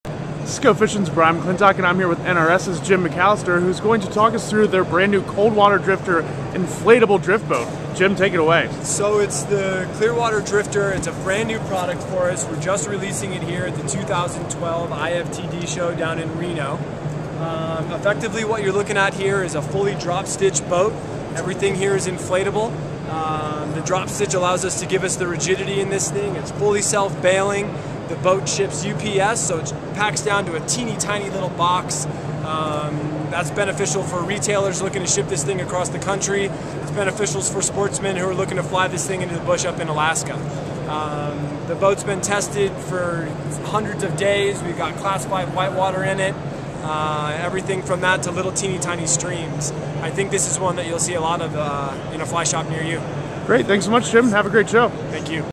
GoFISHn Live at IFTD 2012. The NRS Cold Water Drifter
A drift boat that ships UPS? That's what the NRS Cold Water Drifter is. GoFISHn's Brian McClintock chatted with Jim McCallister about this innovative, inflatable drift boat at the 2012 International Fly Tackle Dealer Show in Reno, Nevada.